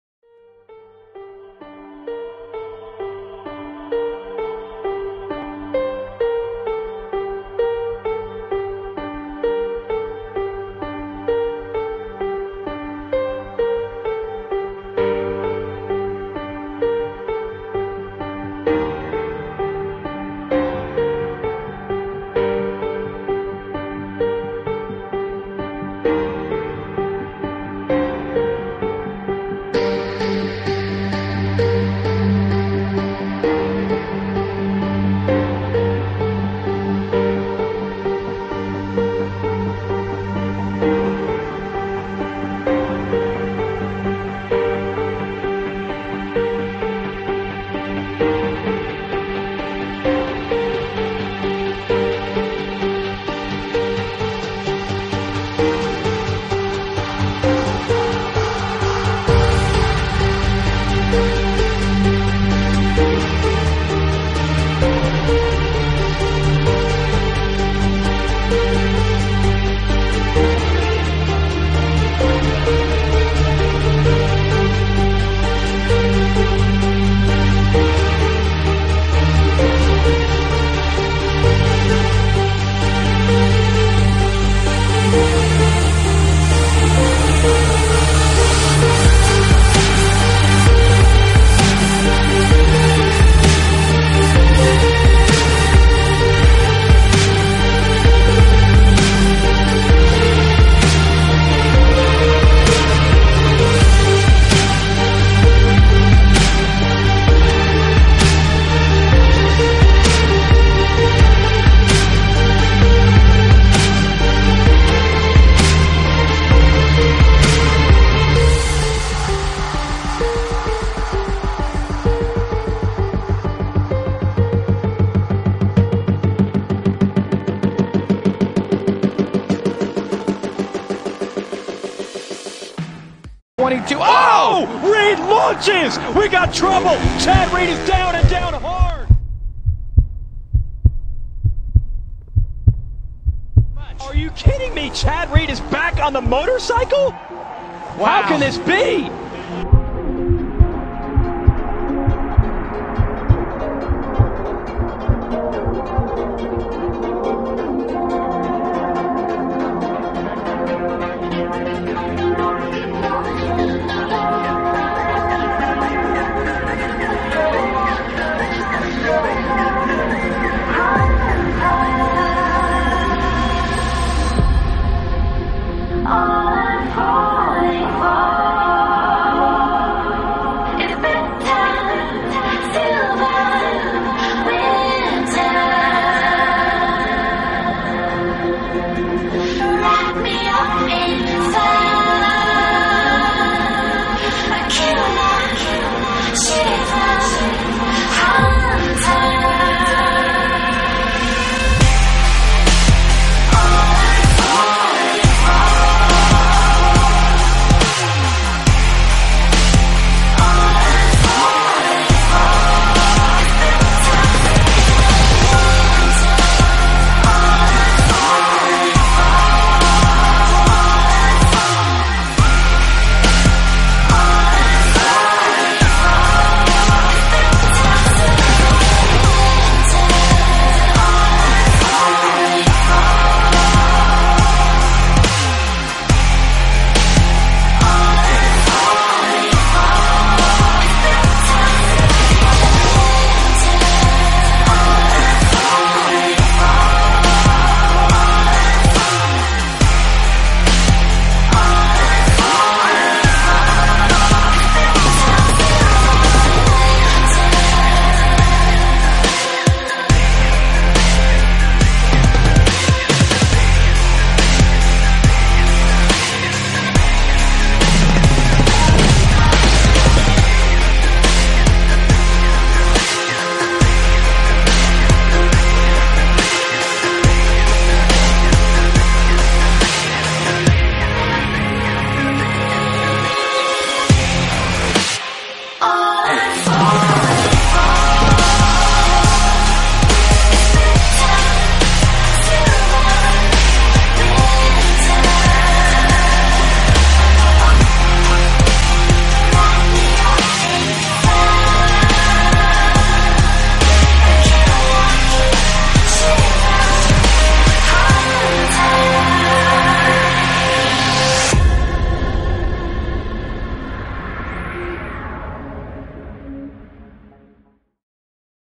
The ama Moto Cross life
this is a video of what some riders have to eventually go threw to become pro